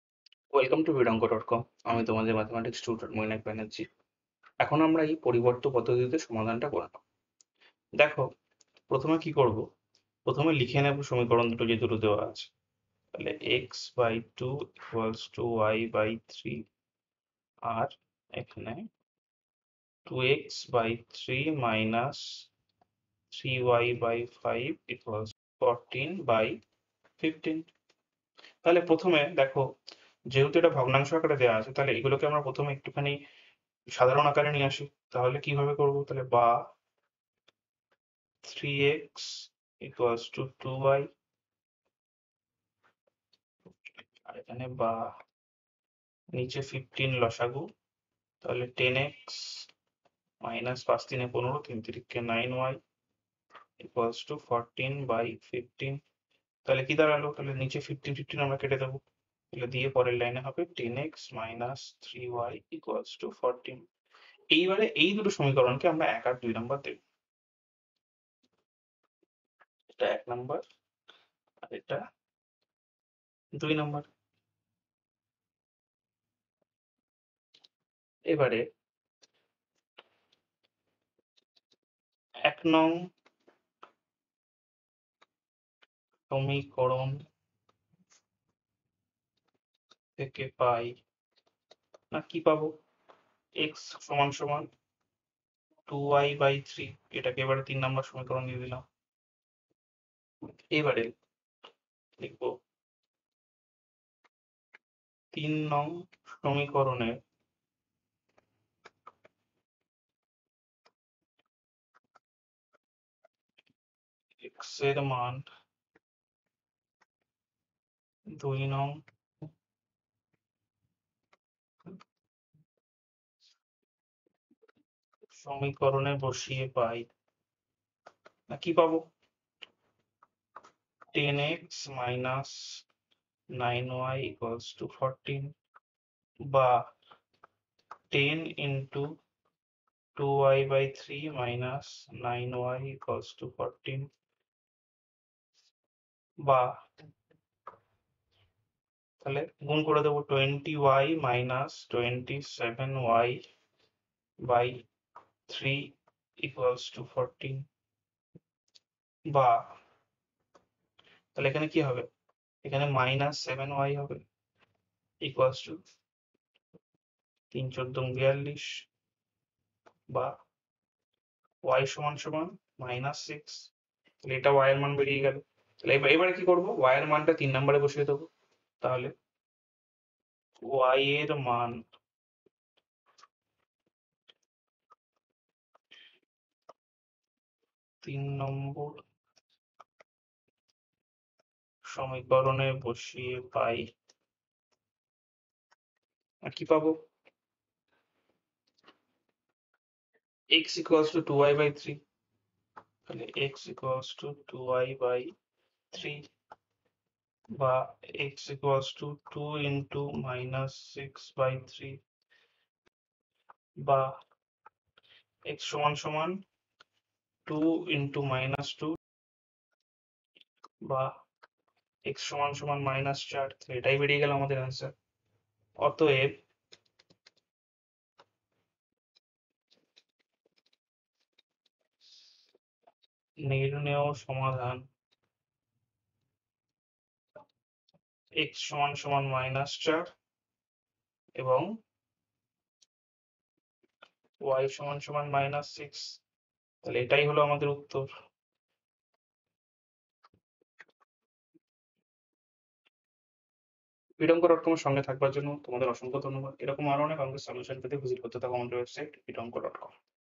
WB Board Class 9 Mathematics Book Solution in Bengali - Amitava Mitra Exercise Question: 4.12(ii)
Get free Maths book video and PDF solutions in Bengali for Amitava Mitra. Free answer and explanation through video for question 4.12(ii), of Chapter Linear Simultaneous Equations from Amitava Mitra followed for Class 9, West Bengal Board of Secondary Education (WBBSE).